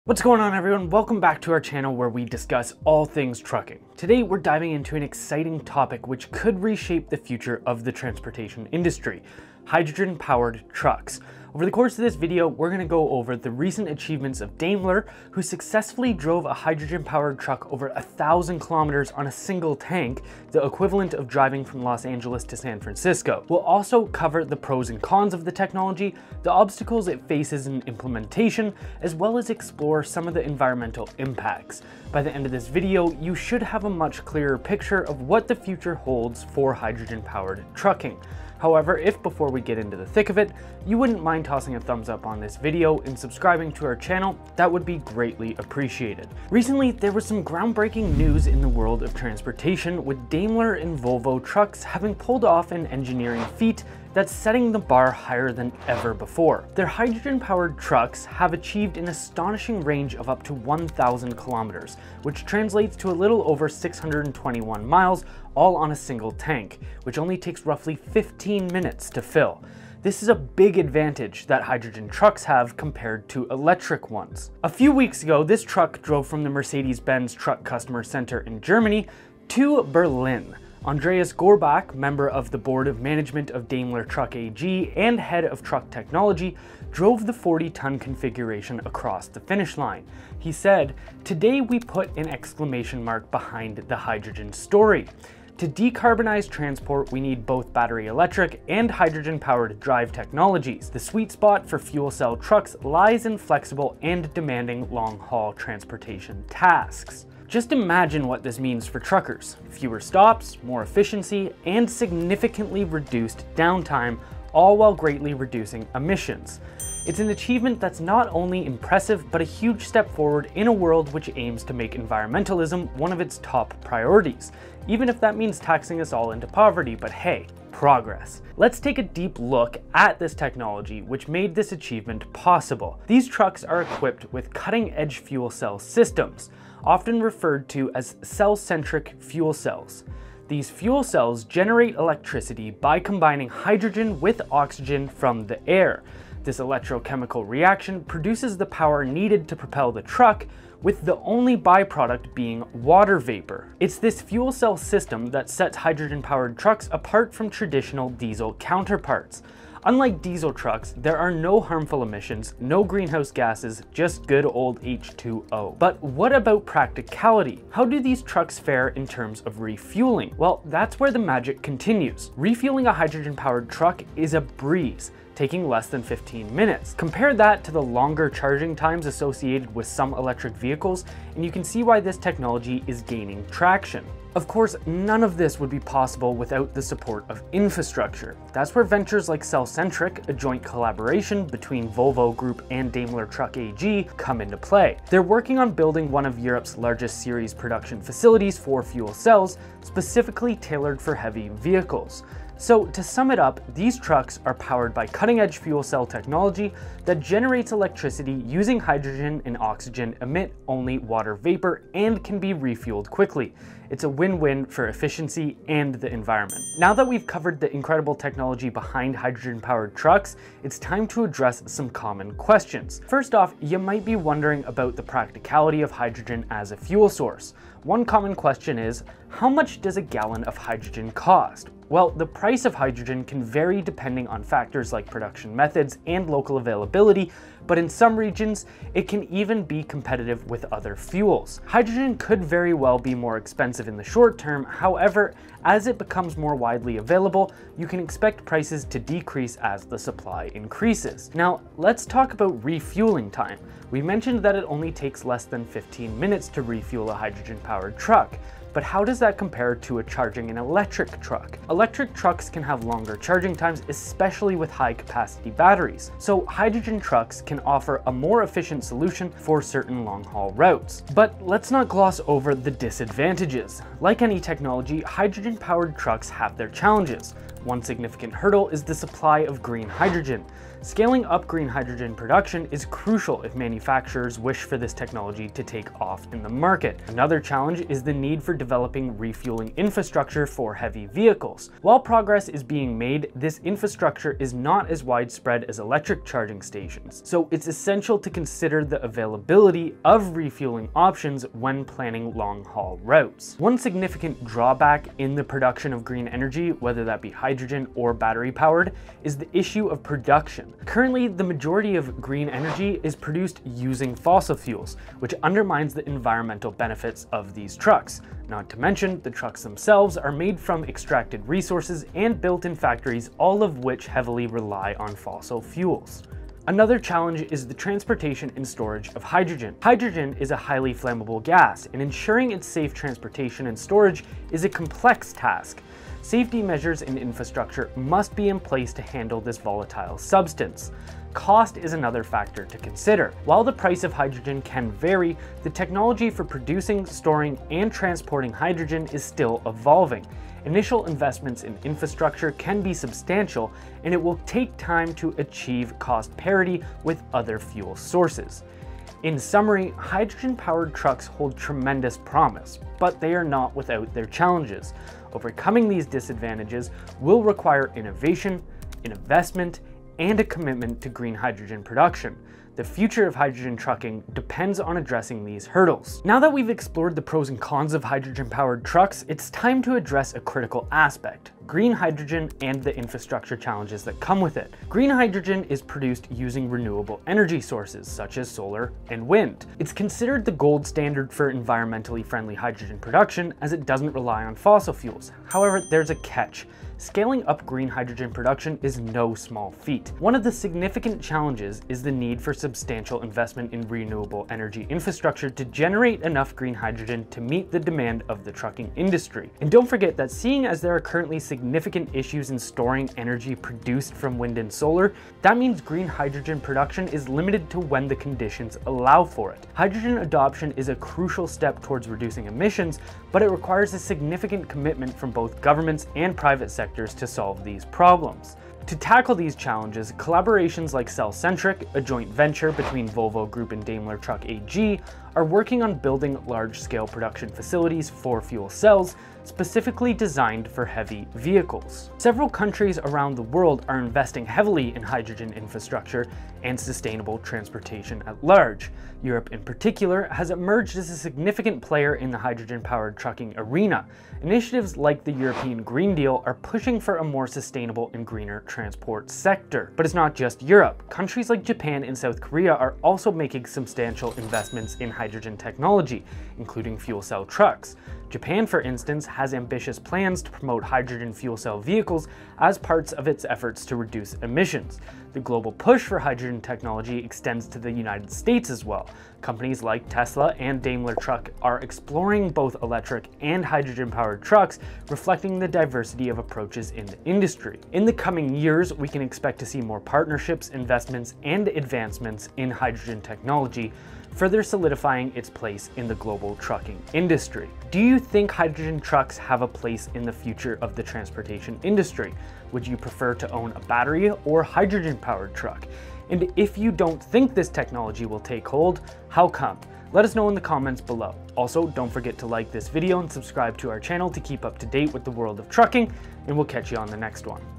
BREAKTHROUGH With Hydrogen Powered Trucks | 1000 km on ONE Tank!
Hydrogen trucks are on the rise!!

In this video, Liam shares some of the major breakthroughs that have recently occurred in the world of hydrogen trucks, specifically with Daimler and their 1000km journey on a single tank of hydrogen!

He discusses what we can expect to see more of with the increasing development of this industry, how they function, and how they will progress in the near future.

What do you think of this technology and it's recent developments?

Chapters 📚:

00:00 Introduction- Hydrogen Powered Trucks
00:52 Daimler & Volvo Hydrogen Trucks Breakthrough
01:09 15 minutes to fill hydrogen tank in semi truck
01:19 Daimler hydrogen truck drove 1000km with single hydrogen tank
02:13 Looking into hydrogen truck technology
02:23 Cellcentric fuel cell science
02:40 Hydrogen compared to diesel trucks
02:54 Refuelling a hydrogen truck
03:13 Building infrastructure for hydrogen trucks
03:51 How much does a gallon of hydrogen cost?
04:20 Electric trucks can have longer charging times than hydrogen trucks
04:37 Challenges of hydrogen trucks
05:13 Green energy production setbacks
05:27 Transportation and storage of hydrogen
06:23 Challenges with green hydrogen production
07:38 European green deal
07:48 Japan's plans for promoting hydrogen vehicles